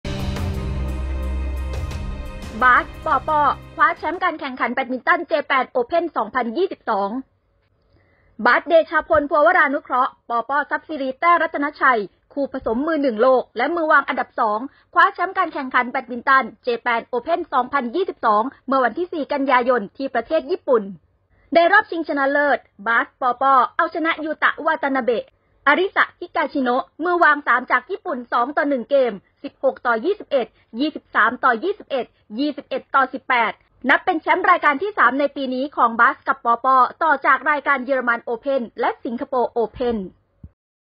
“บาส-ปอป้อ” คว้าแชมป์โยเน็กซ์ ไดฮัทสุ เจแปน โอเพ่น 2022
"บาส” เดชาพล พัววรานุเคราะห์ กับ “ปอป้อ” ทรัพย์สิรี แต้รัตนชัย แชมป์โลก 2021 คู่มืออันดับ 1 ของโลก แซงกลับมาเอาชนะ รองแชมป์โลก 2 สมัยอย่าง ยูตะ วาตานาเบ้ กับ อลิสะ ฮิกาชิโนะ  คู่มืออันดับ 3 ของโลกจากญี่ปุ่น ไป 2-1 เกม 16-21 ,23-21 และ 21-18

"บาส” เดชาพล กับ “ปอป้อ” ทรัพย์สิรี คว้าแชมป์แบดมินตันโยเน็กซ์ ไดฮัทสุ เจแปน โอเพ่น 2022 ทัวร์นาเมนต์ระดับเวิลด์ทัวร์ 750 ที่เมืองโอซาก้า ประเทศญี่ปุ่น เมื่อวันที่ 4 ก.ย.65

ภาพ Badmintonphoto